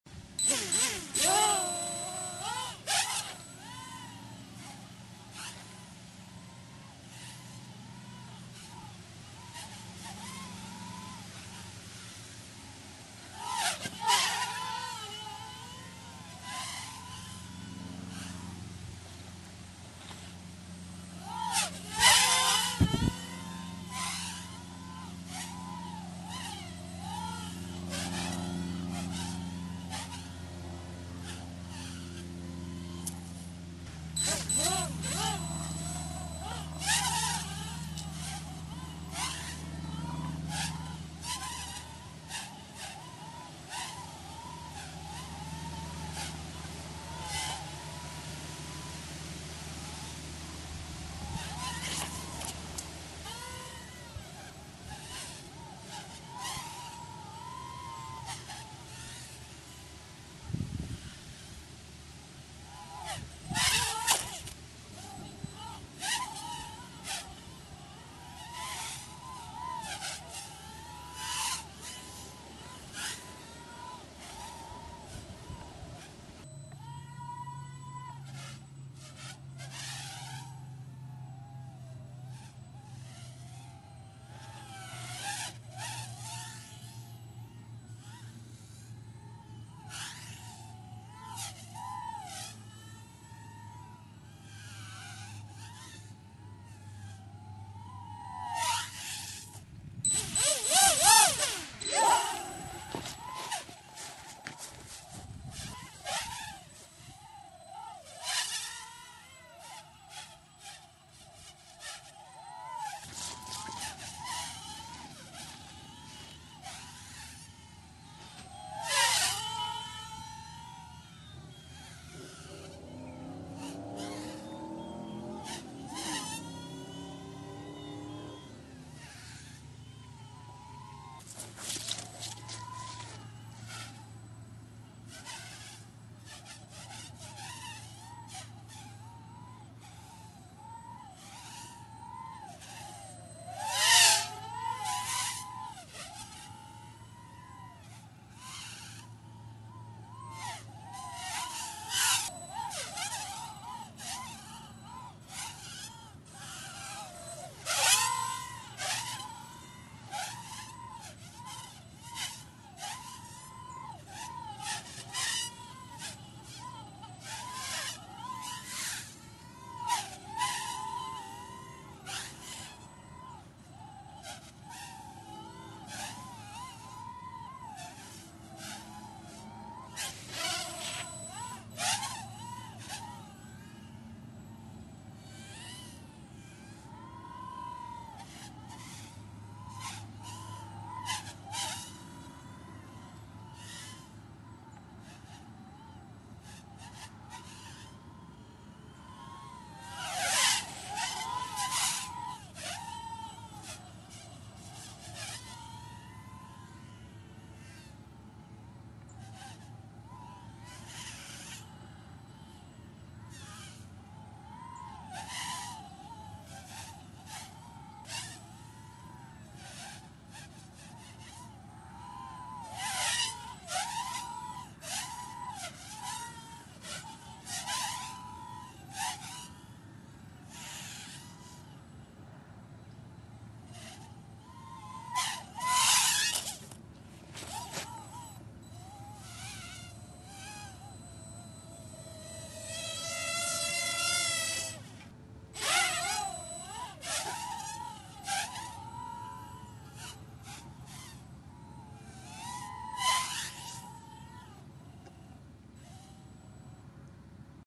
Blade nQX controlled quadcopter flying acro with flips rolls speed and crashes
Another nQX controlled micro quadcopter I put together.  Constructed using V959 motors, cut down v949 motor holders, a Hisky FF120 frame, 3x3 carbon booms and the nQX flight controller.   Flown mostly with a 300mah turnigy nano-tech battery.  Flight times are short but this is one acrobatic monster.  Still trying to get a handle on this one.